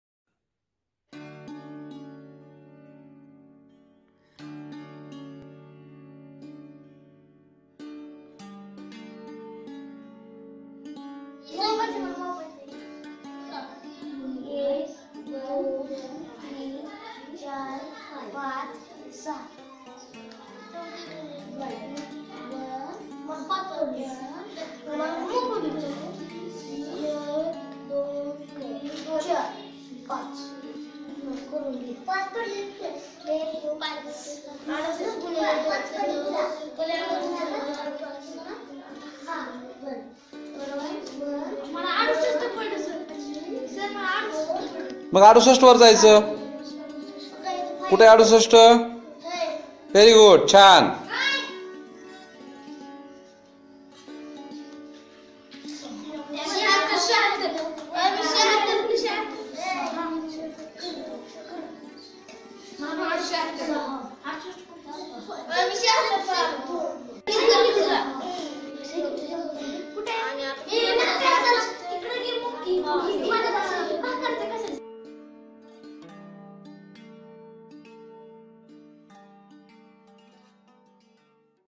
गणित शिडी
गणित शिडी हा खेळ विविध गणिती क्रिया करत मुले खेळू शकतात.